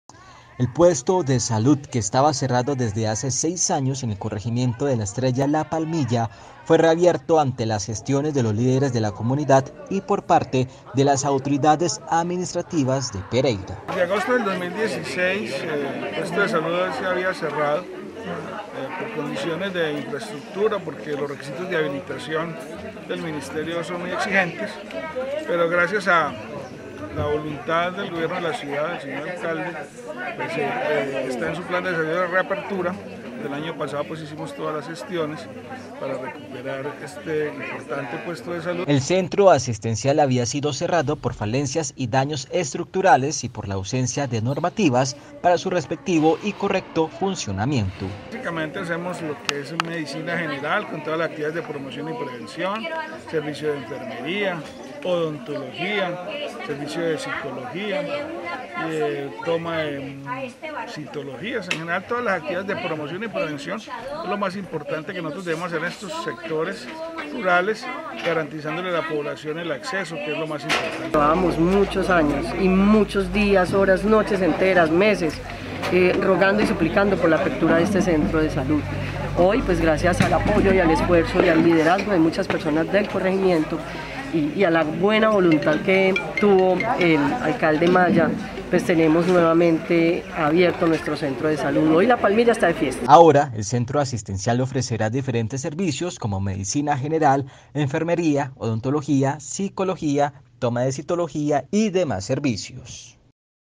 FUE REABIERTO EL CENTRO ASISTENCIAL LA ESTRELLA – LA PALMILLA
El puesto de salud que estaba cerrado desde hace seis años en el corregimiento de la Estrella – La Palmilla, fue reabierto ante las gestiones de los lideres de la comunidad y por parte de las autoridades administrativas de Pereira.